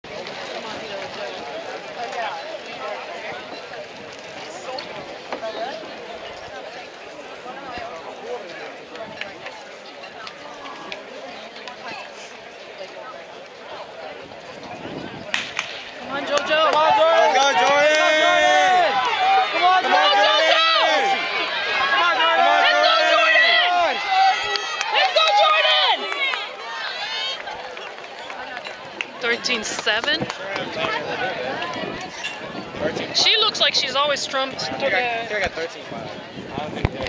March 19 2022 Jojo 100m Season best
Jordyn Hairston ties her PR in the 100m dash at the Clash of the Carolinas!